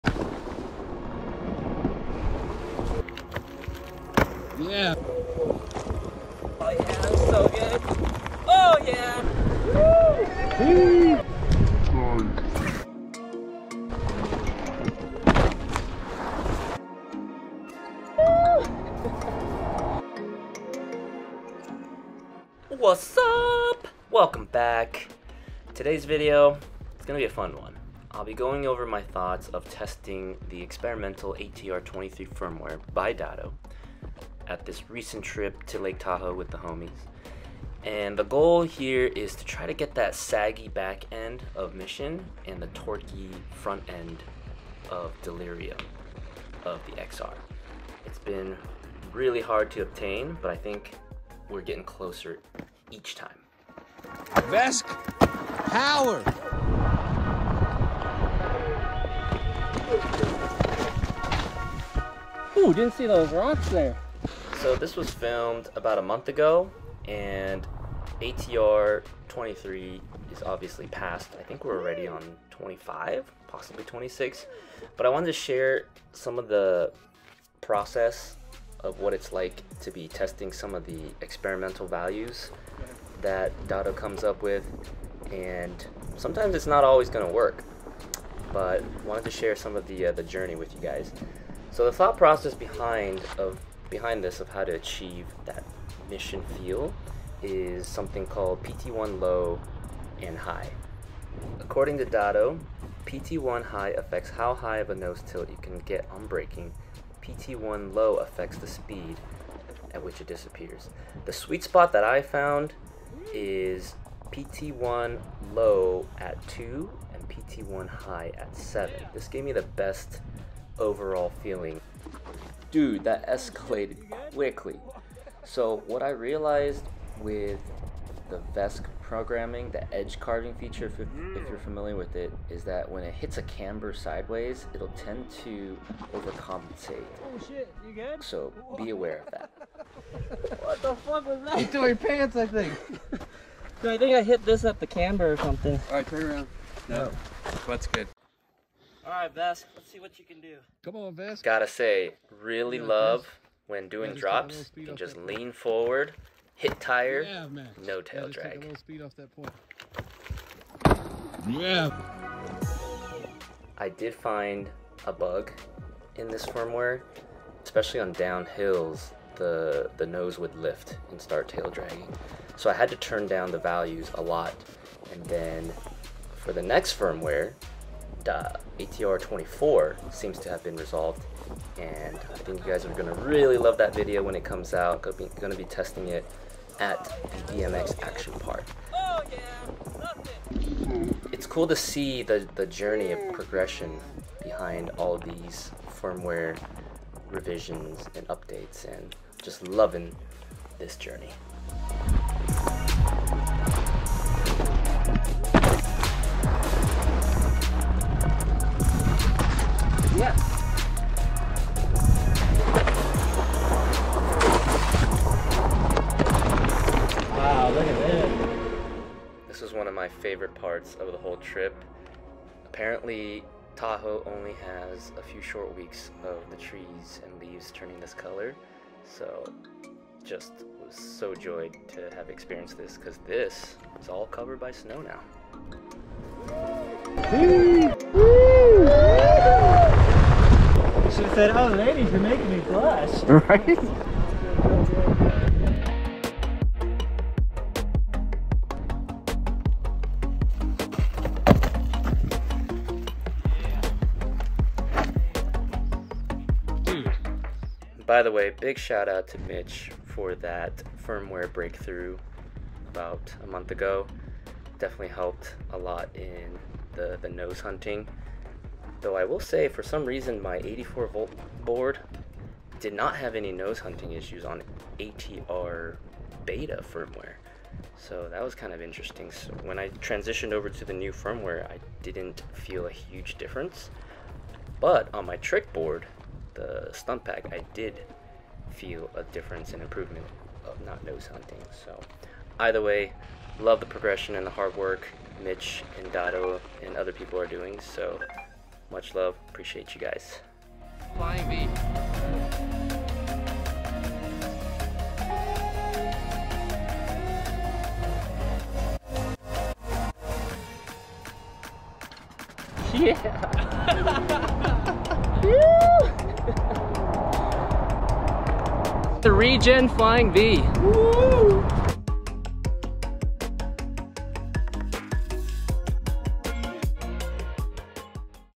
Testing ATR 23 Firmware (experimental) for VESC at Tahoe!
Join the process of trying new features for VESC development! The goal for this ATR 23 firmware from Dado is to try and get closer to that XR feeling that Jeff McCosker so desperately wants: saggy backend of Mission and torquey front end of Delirium.

It doesn't always work though, have to be patient and get out of your comfort zone to try new things for the sake of feedback for R&D.. Shoutout to Dado @surfdado for thinking out of the box, Mitch Lustig for the breakthrough IMU correction, and Dave Stewart for starting (VESC resource forum).

Chapters:
00:00 Intro
01:33 The pursuit of 'Mission' via Pt1 low/hi
02:43 Found a bug!
03:38 Golden Tahoe
04:18 Shoutout Mitch & Dado